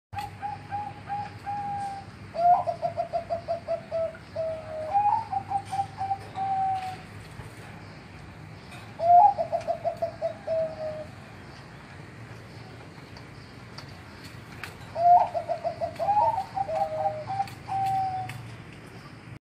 Morning BF Yogyakarta Indonesia
Perkutut suara tebal besar sedikit cepat dan cowong